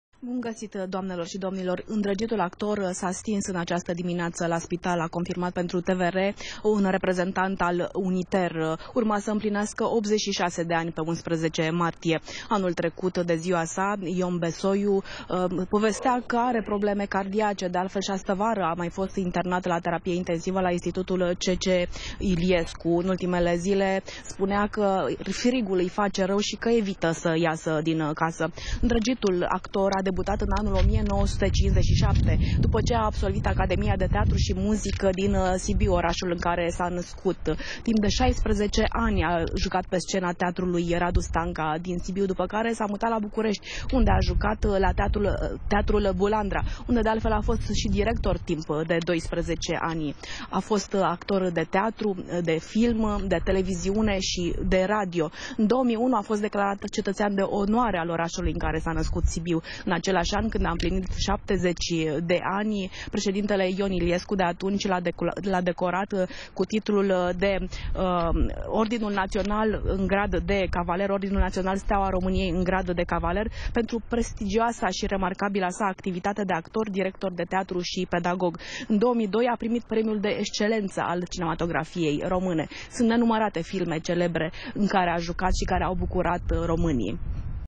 Actorul Ion Besoiu a încetat din viaţă la 85 de ani
Marele actor Ion Besoiu a murit  în această dimineaţă, la Institutul pentru Boli Cardiovasculare C.C. Iliescu. Avea 85 de ani. Ion Besoiu a fost omogiat cu două premii pentru întreaga activitate din partea Uniunii Teatrale din România şi la Gala Gopo.